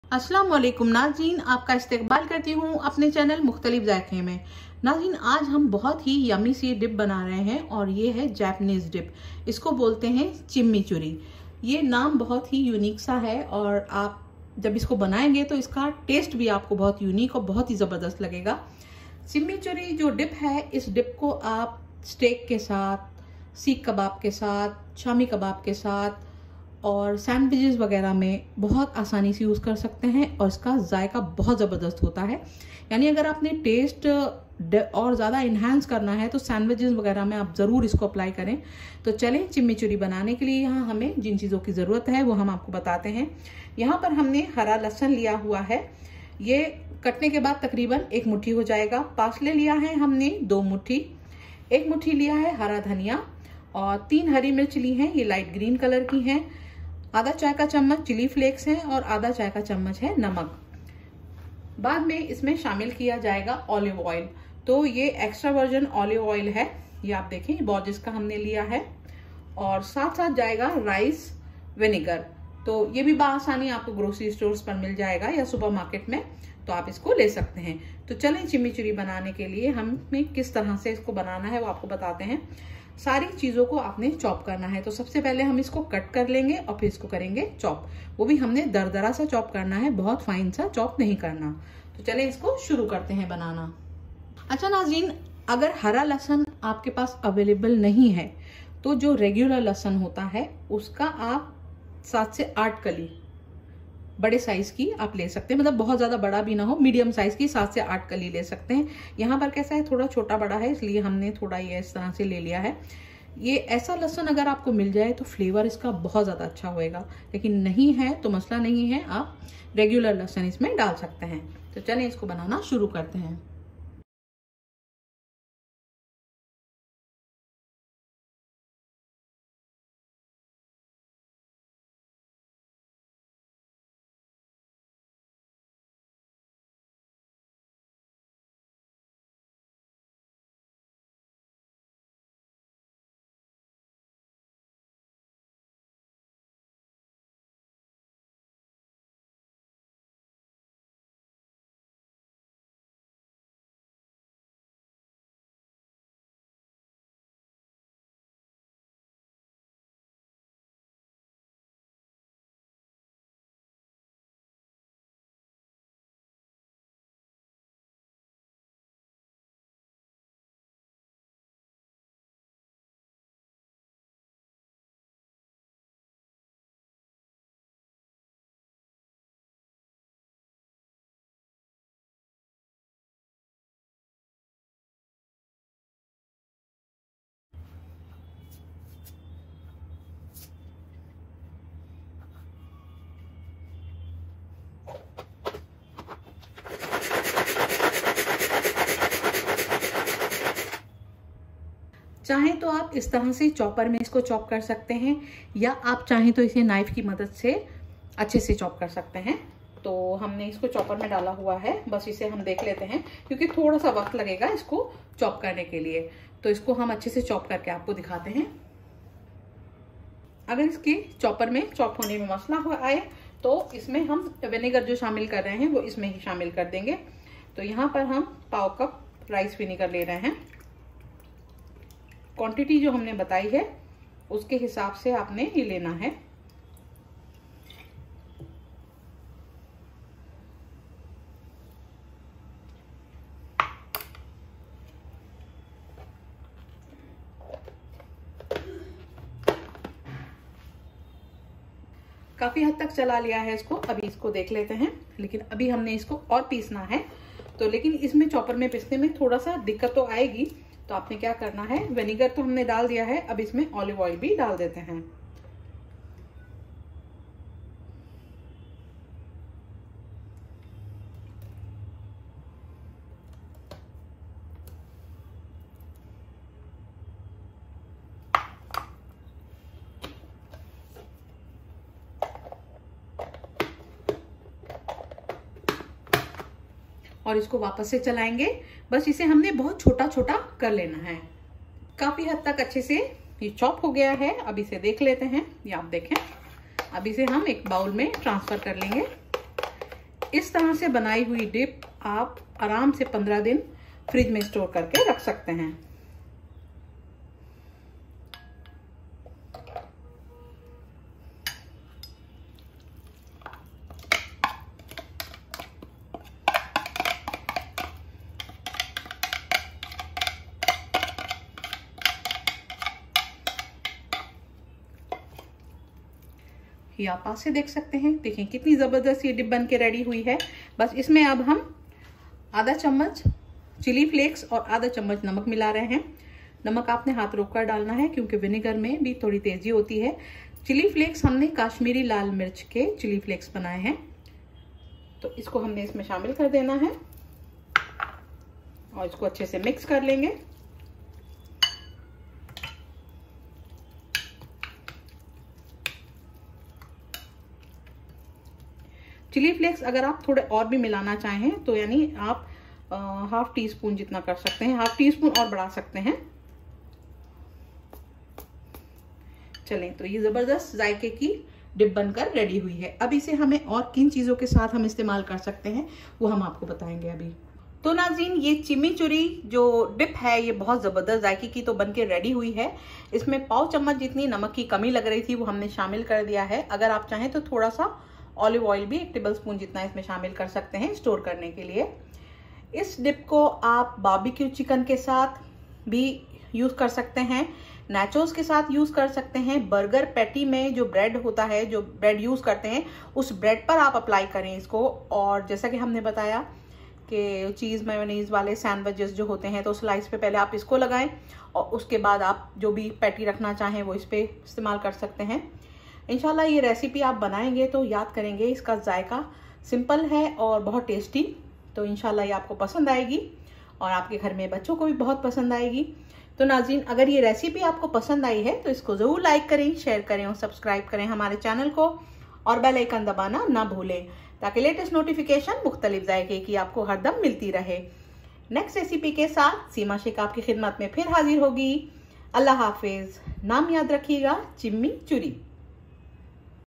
How to make Chimichurri Sauce |Chimichurri dip| Best sauce for Steak | Mukhtalif zaayeke
➤ Chimichurri sauce ya dip easy bhi hai or bahut ziyada tasty bhi is sauce sey aap ke sandwiches , Burger ,Steaks or Kebabs ka taste bahut barjaeyga is ko zaroor try kareyn  15 days glass ki bottle main aram sey fridge main store ker saktey hain